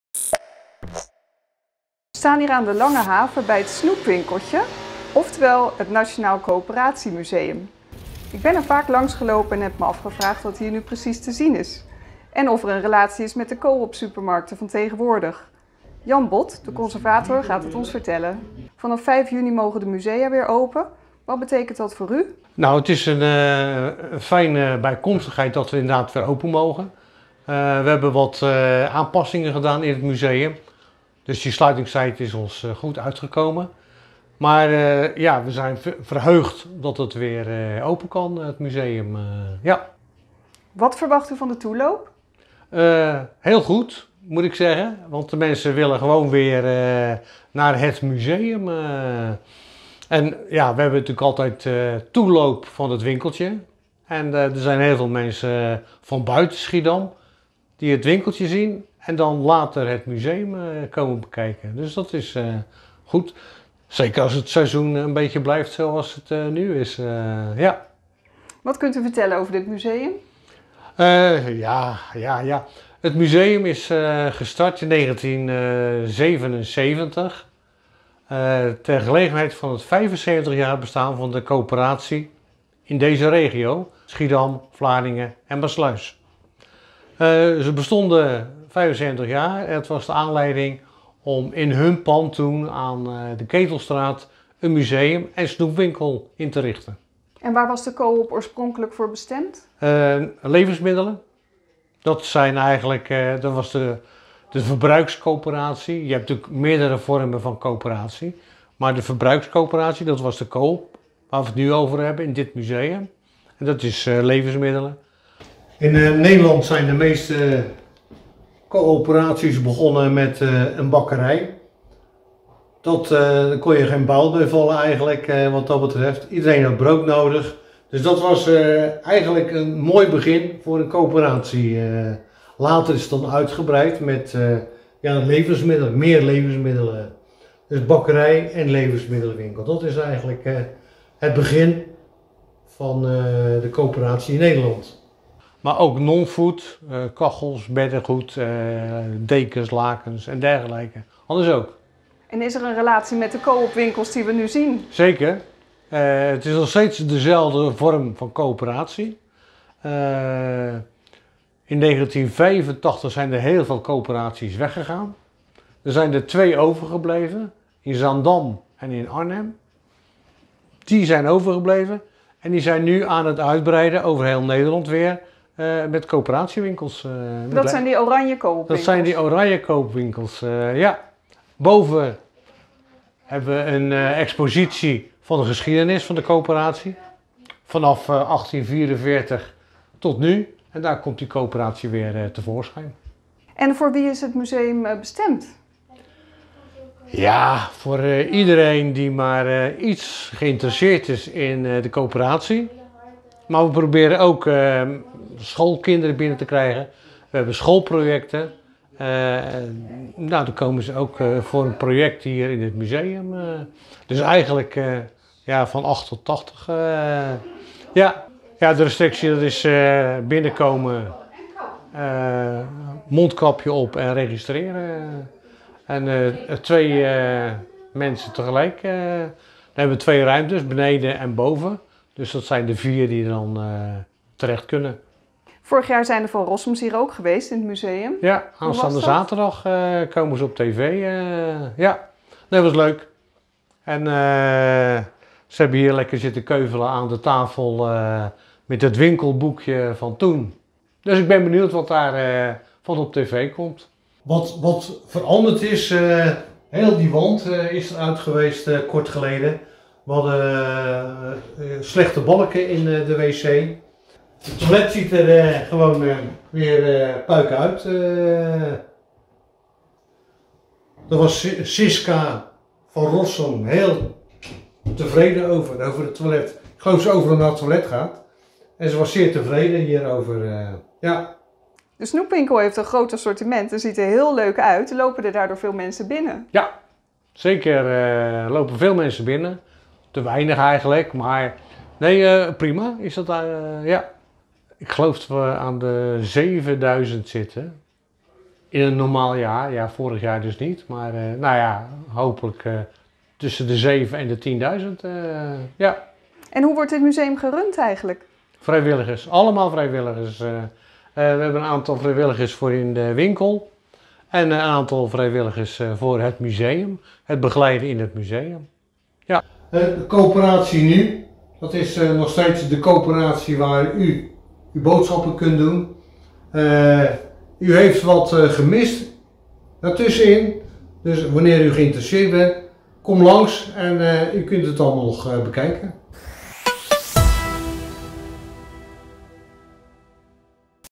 SCHIE TV: Ook het Nationaal Coöperatiemuseum gaat weer open. Ook te zien bij de Van Rossems!
Het Nationaal Coöperatiemuseum in Schiedam gaat ook weer open. Onlangs  waren hier de Van Rossems te gast.